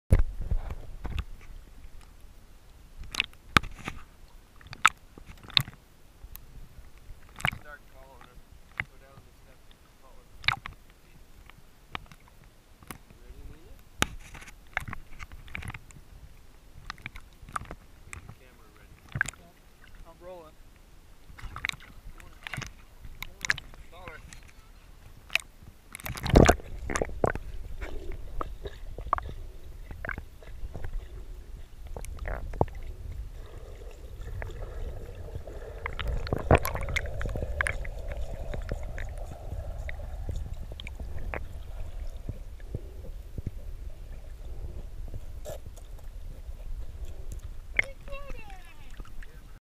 Nina the Shih Tzu swims in the lake
Shih Tzu, Nina, takes a swim in the lake